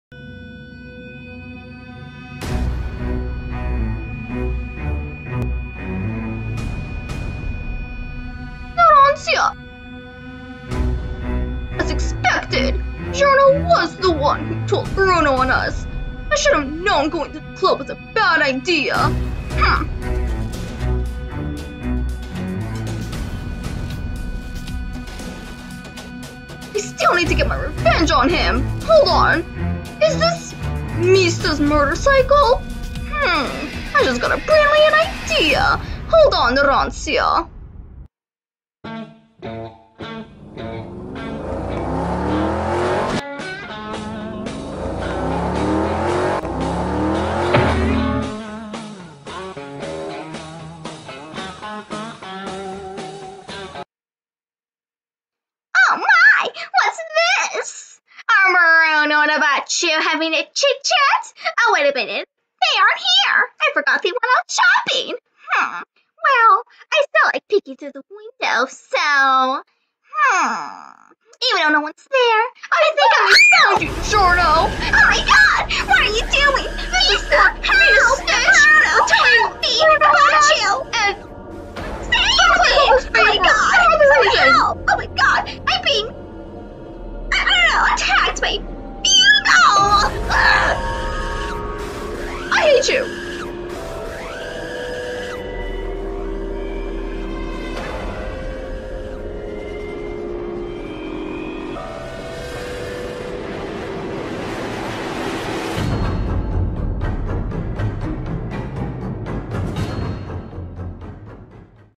Fugo's revenge
Finally brought Fugo's voice back.

Fugo steals Mista's motorcycle to use it against Giorno the Snitch